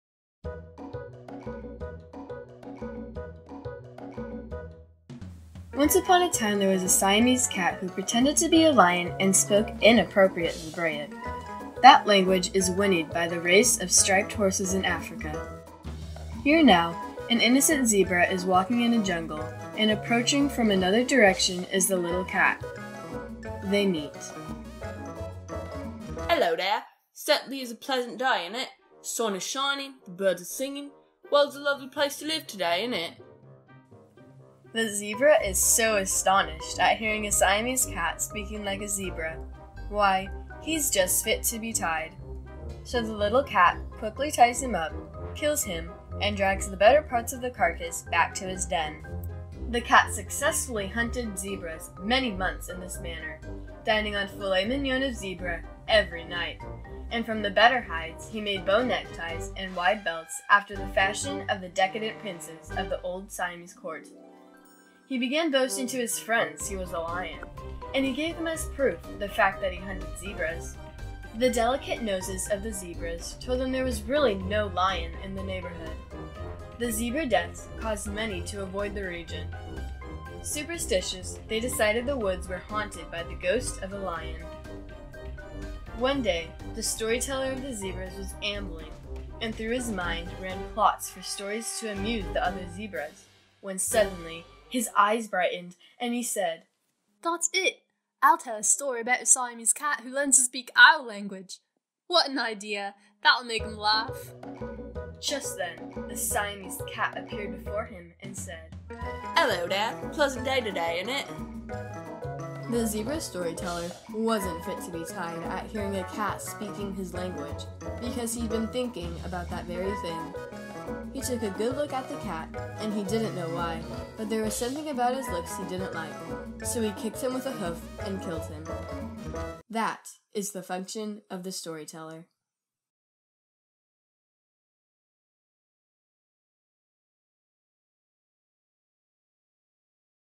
the zebra storyteller
research project for school based on "the zebra storyteller" short story by spencer holst